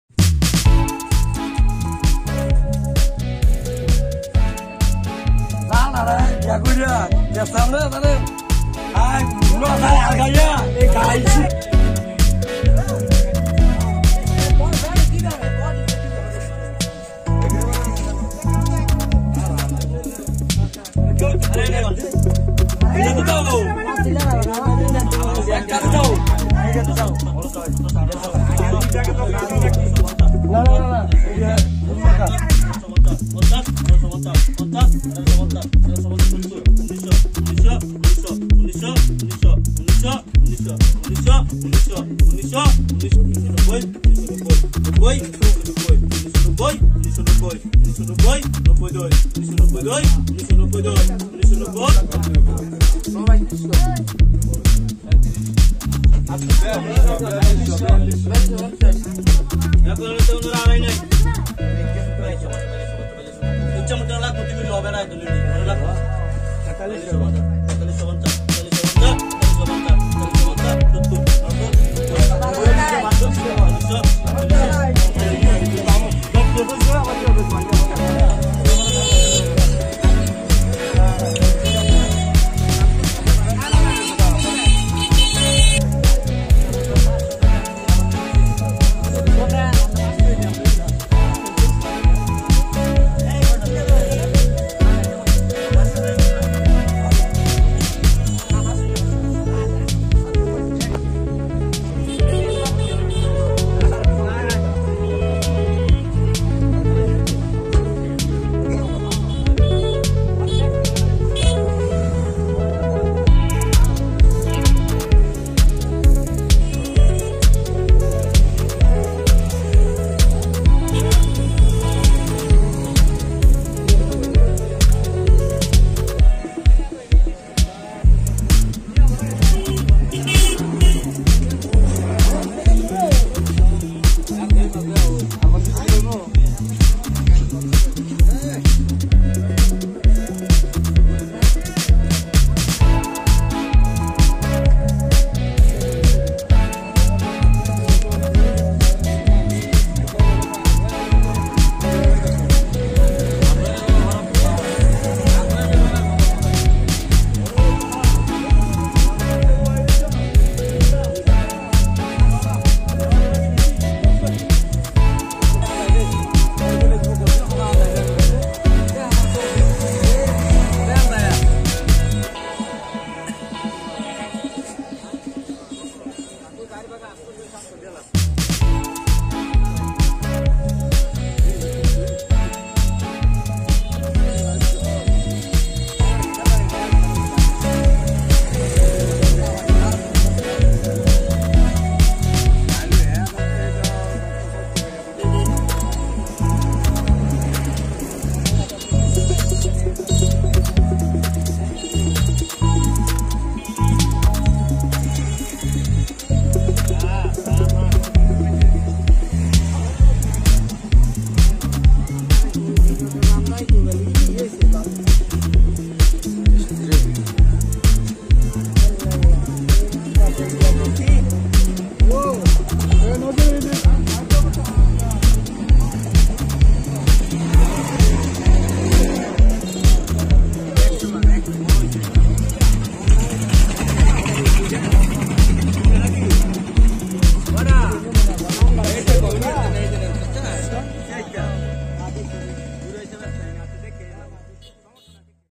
Big Hilsa Fish Magna river মেঘনা নদীর পাড়ে তরতাজা ইলিশ মাছের ‌নিলাম দেখুন
ধন্যবাদ সবাইকে আমাদের পাশে থাকার জন্য //